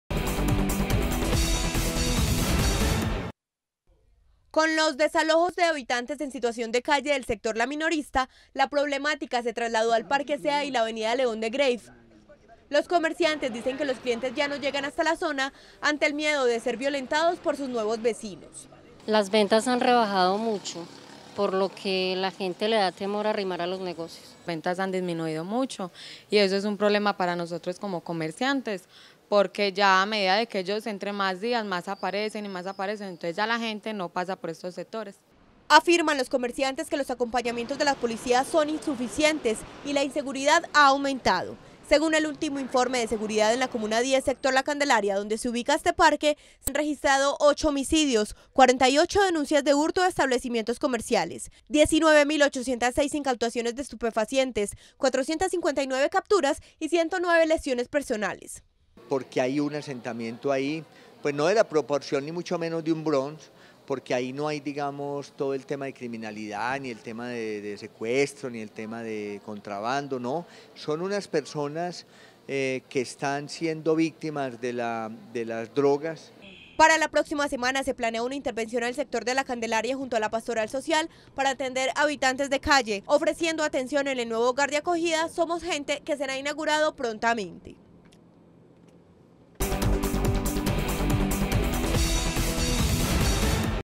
Comerciantes de la Avenida de Greiff han dejado de vender un 70%
Habitantes del sector del Parque Zea y la Avenida León de Greiff, afirman que en un 70% se han reducido las ventas, luego de la llegada de habitantes de calle al sector.

-Teleantioquia Digital-